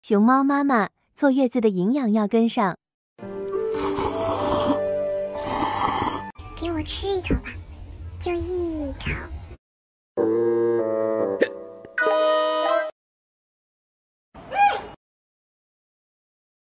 Panda Mama: Essential Postpartum Nutrition.
In this wonderful video, we dive deep into the world of essential postpartum nutrition with Panda Mama! Discover the best foods to nourish your body after giving birth and maximize your energy levels as a new mom. Don't miss out on this valuable information! Join the PandaEnergyCommunity today!